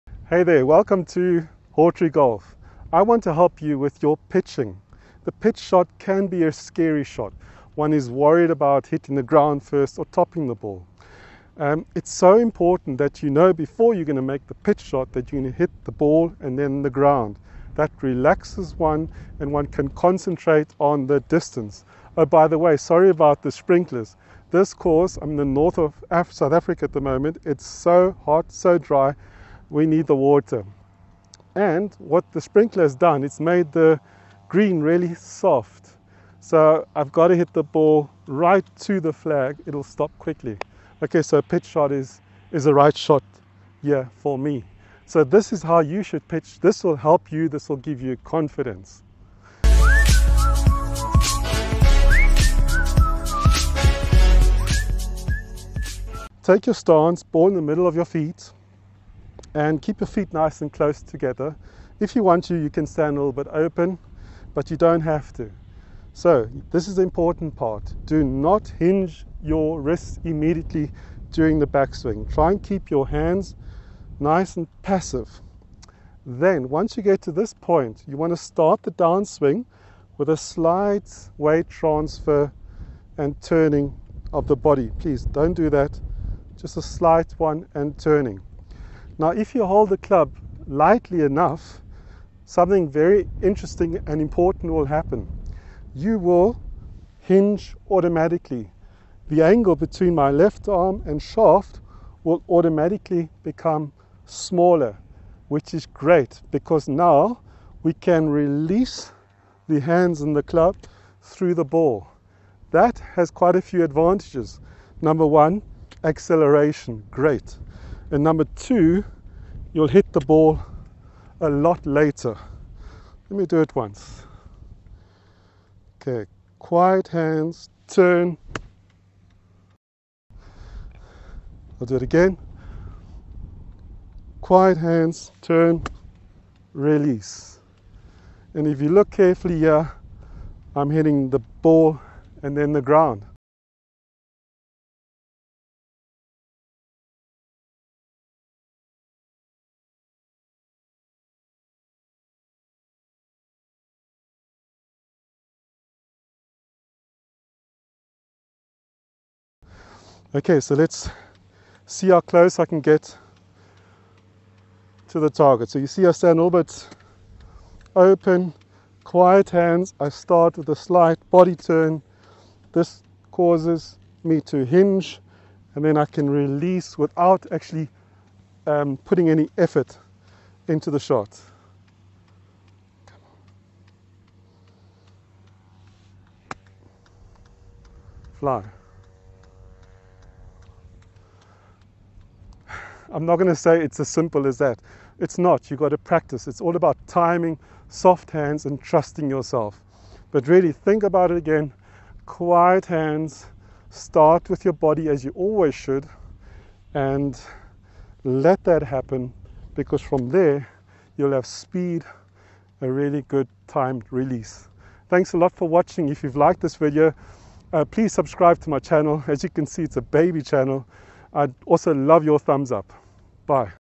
Hit Crisp Pitches with this Natural Technique
The pitching technique shown in this video really helps me (and will help you too) to hit crisp short game shots. This is because it allows me to accelerate and hit the ground after the ball with not much of a divot. The movement is very natural and if you practice it enough, you will get the hand of it and really enjoy it!

Transcript:
0:00 Hey there, welcome to Hawtrey Golf. I want
to help you with your pitching. The pitch
shot can be a scary shot. One is worried about
hitting the ground first or topping the ball.
It so important that you know before your
hit the pitch shot that you are going to hit
the ball and then the ground. That relaxes
one and one can concentrate on the distance.
Oh by the way, sorry about the sprinklers.
This course - I'm in the north of South Africa
at the moment - it's so hot, it's so dry,
we need the water. And what the sprinkler
has done - it's made the green really soft.
So I have got to hit the ball right to the
flag - it will stop quickly. Ok, so the pitch
shot is the right shot here - for me.

0:45 So this is how you should pitch, this is how you should
pitch, this will help you, this will give
you confidence. Take your stance - ball in
the middle of your feet - and keep your feet
nice and close together. If you want to, you
can stand a little bit open, but you don't
have to.

1:06 So, this is the important part. Do
not hinge your wrists immediately during the
backswing. Try to keep your hands nice and
passive. Then, once you get to this point,
you want to start the downswing with a slight
weight transfer and a turning of the body
- don't do that, just a slight one and turn.

1:29 Now if you hold the club lightly enough, something
very important and interesting will happen.
You will hinge automatically. The angle between
my left arm and shaft will automatically become
smaller, which is great because now we can
release the hands and the club through the
ball. That has quite a few advantages: Nr
1: acceleration - great and Nr. 2: you will
hit the ball a lot later.

2:02 Let me do it once.
Quiet hands, turn. I will do it again. Quiet
hands, turn, release. And if you look carefully
here, I am hitting the ball and then the ground.
OK, so let's see how close I can get to the
target. You see I stand a little bit open,
quiet hands, I start with a body turn - this
causes me to hinge and then I can release
without actually having to put any effort
into the shot.

3:13 I'm not going to say it's as
simple as that, it's not, you have got to
practice. It's all about timing, soft hands
and trusting yourself. So really, think about
it again - quiet hands, start with your body
as you always should and let that happen,
because from there you'll have speed and a
really good timed release. Thanks a lot for
watching. If you've liked this video, please

it's a baby channel, I'd also love your thumbs
up. Bye.